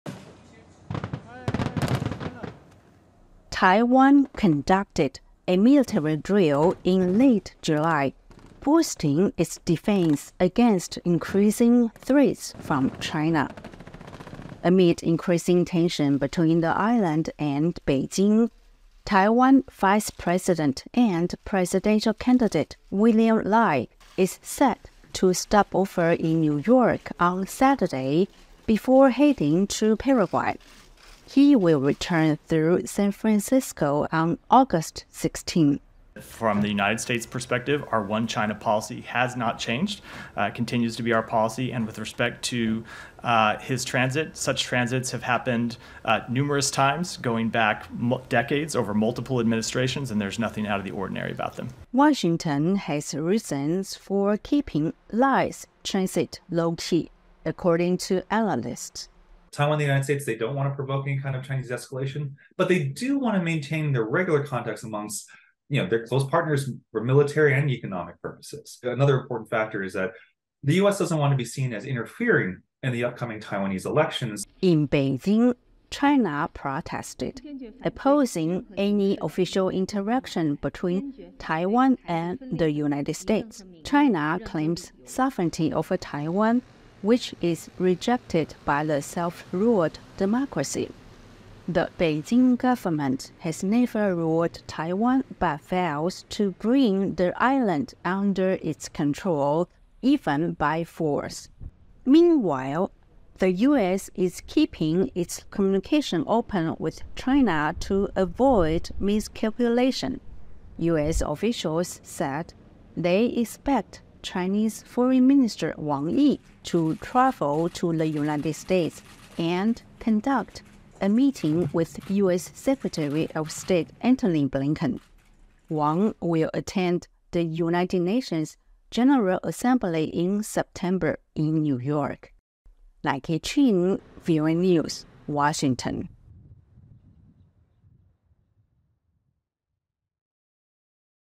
Taiwan VP’s US Stop to Test Already Tense China-US Ties | VOANews
Taiwan’s Vice President William Lai, a front-runner in the island’s 2024 presidential elections, will land Saturday in New York on his way to Paraguay. VOA’s Nike Ching reports on the latest U.S.-China tensions over Taiwan, and the expected trip to Washington by China's foreign minister next month.